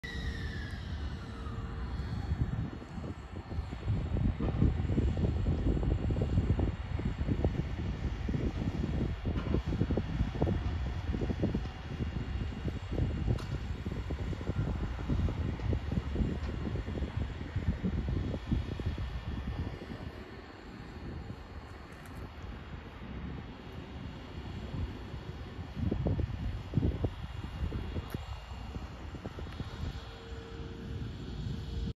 2023年2月22日の仏生山工場
605編成が洗車してました